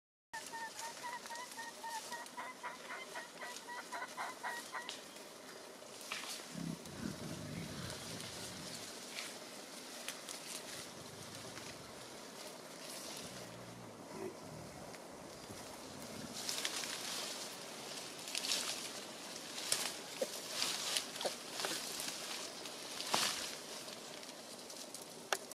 Uwacu Cries Out For Her Mother & Isooko Saves the Day | Dian Fossey Gorilla Fund
From the Fossey Fund vault, watch Uwacu, then 10 months old, call out for her mother. Don't worry, Isooko, 11 years old at the time, saves the day when he picks up Uwacu. 💛

Our videos should not be reposted on YouTube without permission.